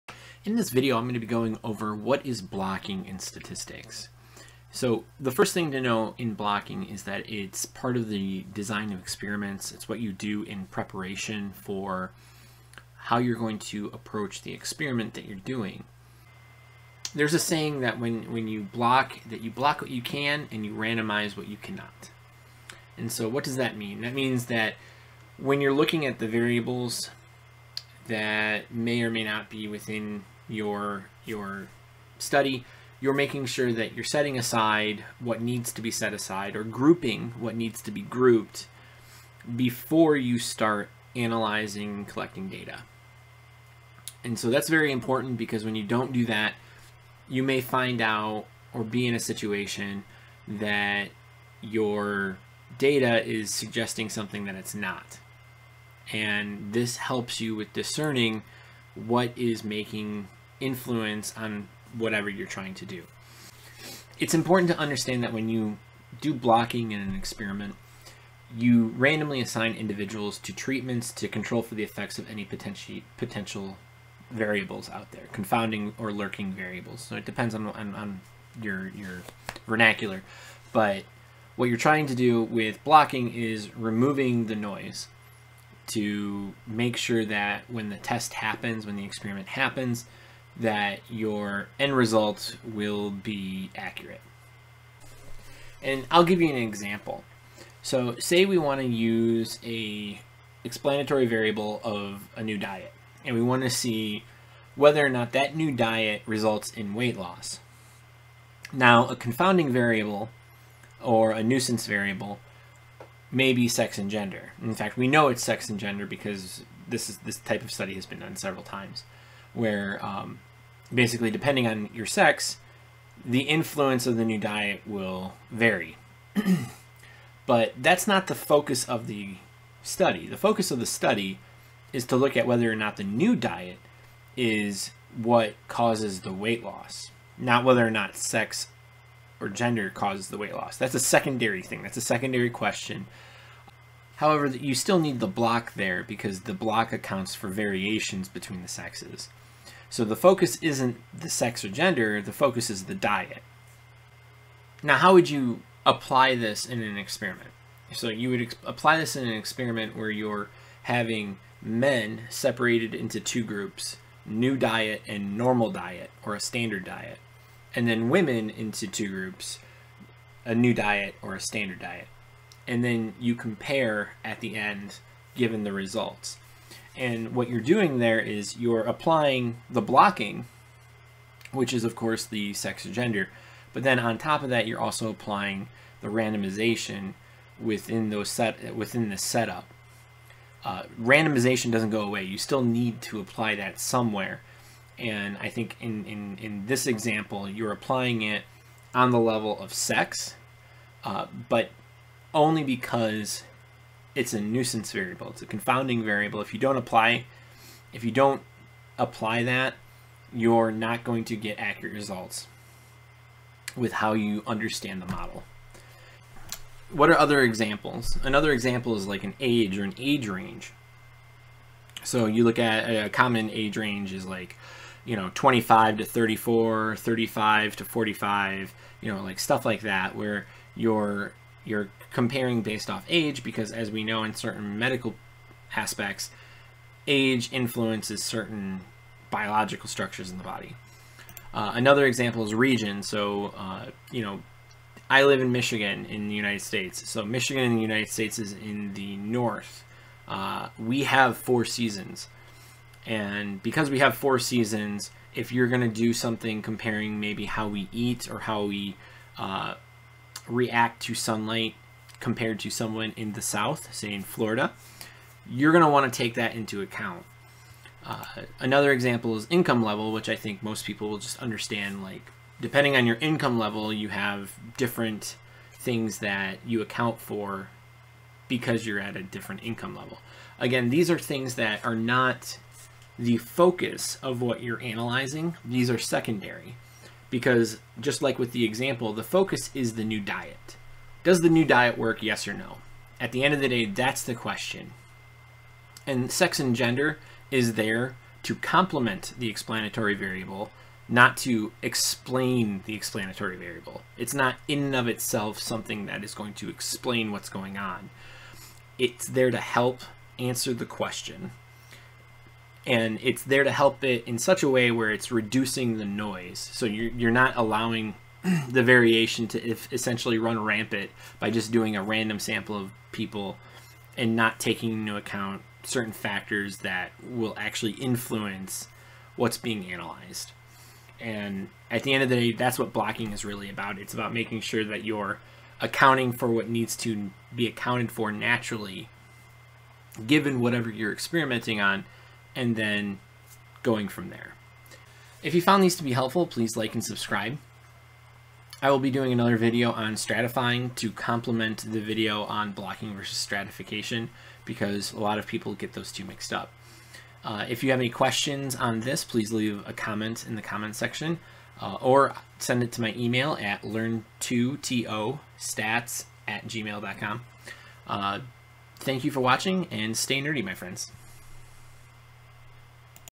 What is Blocking and Confounding Variables in Statistics?
In this video, I go over the design of experiments method of blocking. What is Blocking in Statistics? Blocking is one of experimental design research methods.  I go through an example to show how blocking reduces noise and how confounding variables are taken into account.

Statistics Study Guide:

Prime Student 6 Month Trial: